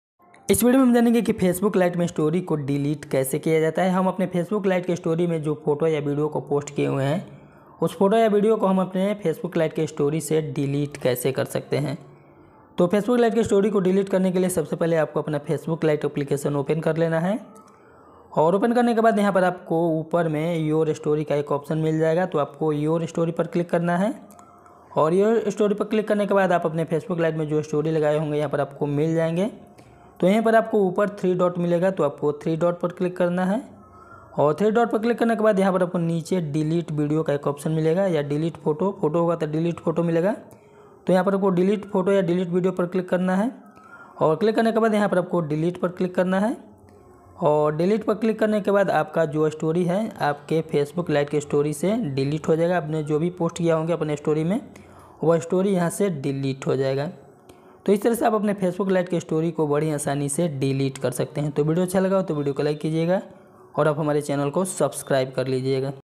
facebook lite se apni story kaise hataye । fb lite se story kaise delete kare
इस विडियो में बताया गया है कि facebook lite se apni story kaise hataye जाते है या fb lite se story kaise delete kare जाते है।